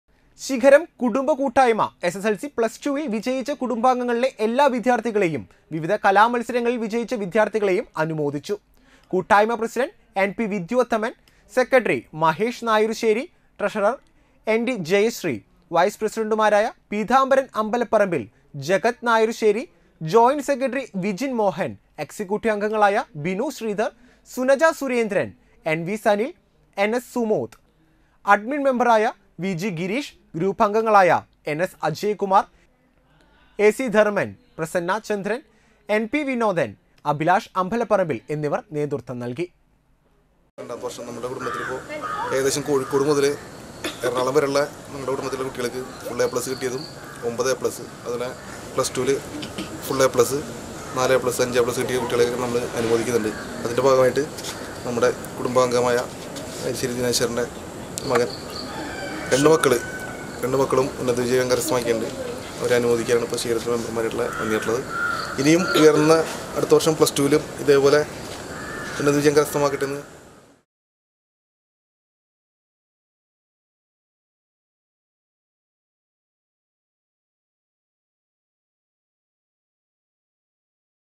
ശിഖരം കുടുംബ കൂട്ടായ്മ വിദ്യാര്‍ത്ഥികളെ അനുമോദിച്ചു.
ശിഖരം കുടുംബ കൂട്ടായ്മ എസ്.എസ്.എല്‍.സി, പ്ലസ് ടുവില്‍ വിജയിച്ച കുടുംബാംഗങ്ങളില എല്ലാ  വിദ്യാര്‍ത്ഥികളെയും വിവിധ കലാ മത്സരങ്ങളില്‍ വിജയിച്ച വിദ്യാര്‍ത്ഥികളെയും അനുമോദിച്ചു.